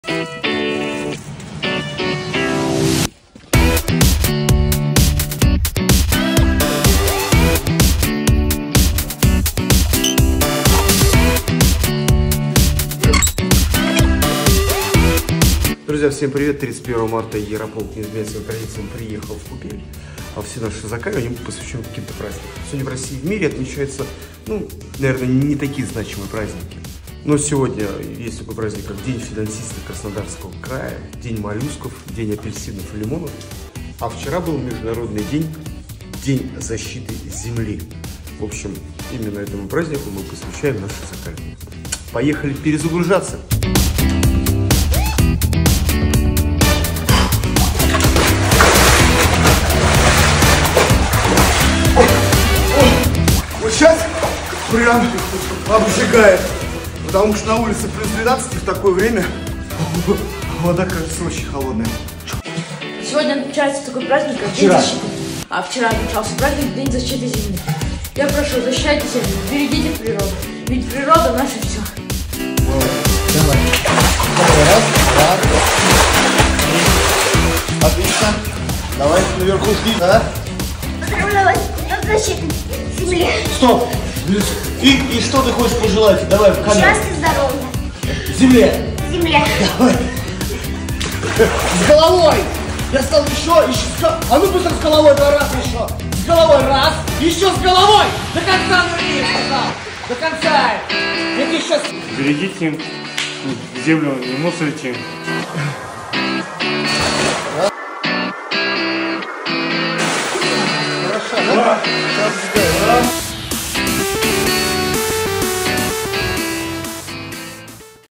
ЗАКАЛИВАНИЕ клуба ЯРОПОЛК / ДЕНЬ ЗАЩИТЫ ЗЕМЛИ / ПОВЫШАЕМ ИММУНИТЕТ, 31.03.2024
Сегодняшнее закаливание мы посвятили Международному празднику ДЕНЬ ЗАЩИТЫ ЗЕМЛИ, который отмечался в России и в Мире  вчера, 30 марта!

Все наши закаливания мы стали посвящать различным праздникам нашей страны.

Ведь культура Российского праздника составляет яркую, очень живую и многообразную сторону жизни жителей нашей страны и, по нашему мнению, это закладывает в сознание спортсмена определённые знания о России.
2. Клуб в яндекс ДЗЕН
3. Канал клуба в РУТУБЕ